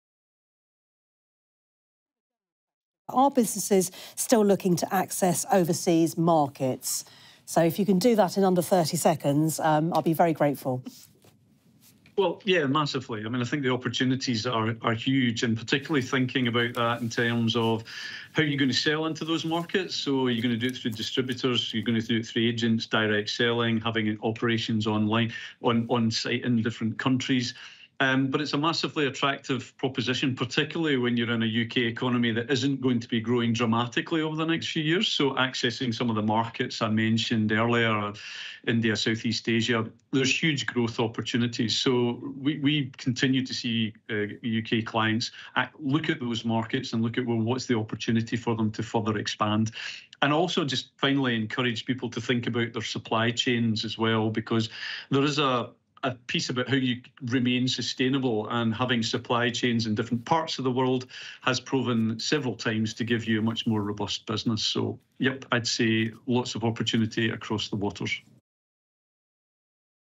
Are businesses still looking to access overseas markets?
Robert Hannah, Head of Large Corporate and Government Advisory at Grant Thornton UK, shares which international markets he thinks UK businesses are looking to for global growth opportunities.